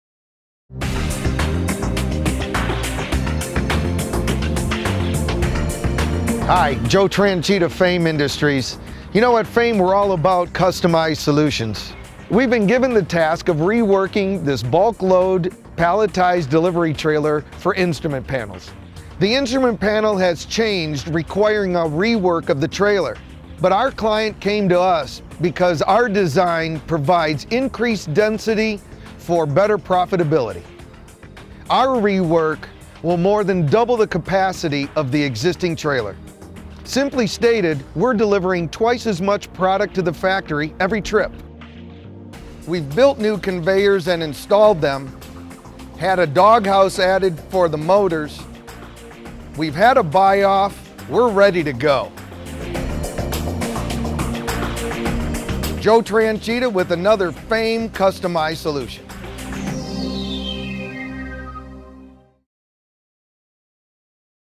Fame Bulk Load Trailers
Fame Industries provides the design and manufacturing of innovative customized material handling solutions. Customized conveyors, automation, ergonomic lift assists and automated guided carts (AGC). This video shows how Fame Industries re-works equipment. These trailers were being used by the customer at another Assembly Plant and delivered to Fame Industries to be re-worked for a new product being installed at another Assembly Plant. The existing conveyors were removed and scrapped. New conveyors were designed, manufactured and installed into the stripped trailers.